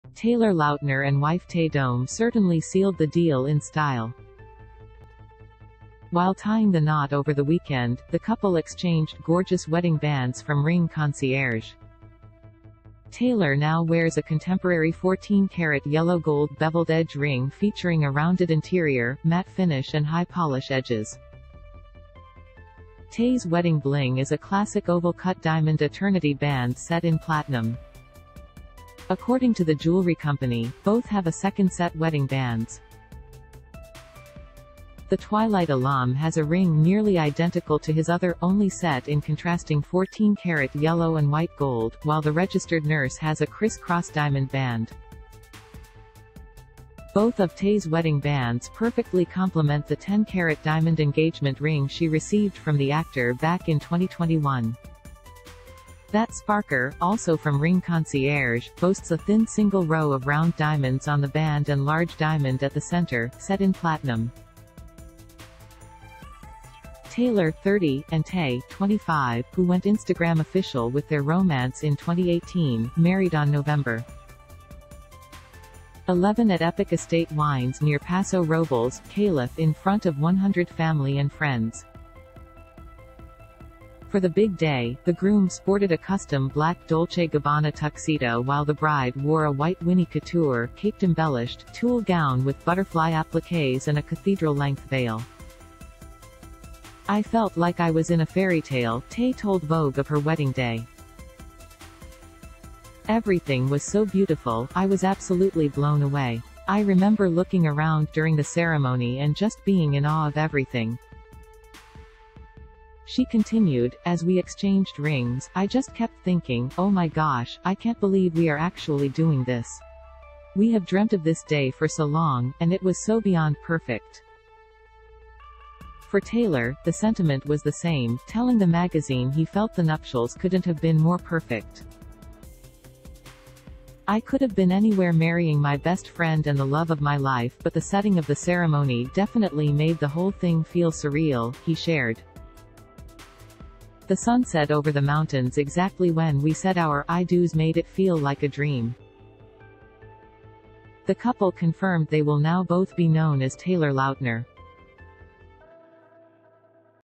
All the Sparkling Details on Taylor Lautner and Tay Dome’s 10-Carat Diamond Ring and Wedding Bands -
Taylor Lautner and wife Tay Dome certainly sealed the deal in style.  While tying the knot over the weekend, the couple exchanged gorgeous wedding bands from Ring Concierge. Taylor now wears a contemporary 14 karat yellow gold beveled edge ring featuring a rounded interior, matte finish and high-polish edges. Tay's wedding bling is a classic oval cut diamond eternity band set in platinum.  According to the jewelry company, both have a second set wedding bands. The Twilight alum has a ring nearl